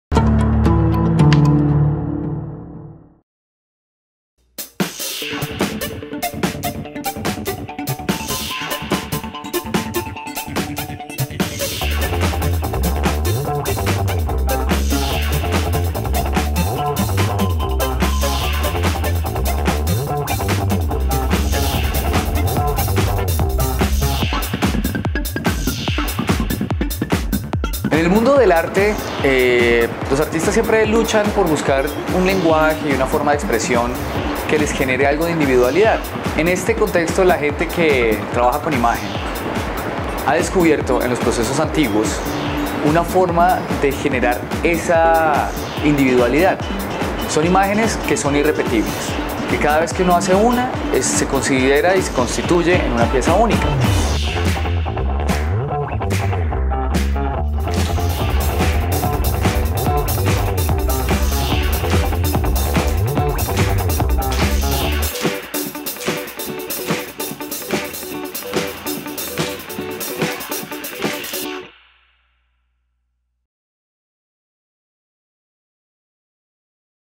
Taller de Procesos Antiguos en Zona Cinco
El profesor Mario Nieto explica cómo, por medio de un aprendizaje lúdico, logramos entender y elaborar imágenes a través de algunos de los procesos que precedieron a la fotografía análoga y que brindaron su aporte en el desarrollo tecnológico de la consecución de imágenes a través de la luz.

Rescatando estos procesos de los anales de la historia, se contribuye en la difusión de este saber antiguo que además hoy en día es bastante acogido por el exclusivo nicho de los artistas fotográficos.

REALIZACIÓN
Erick Casanova G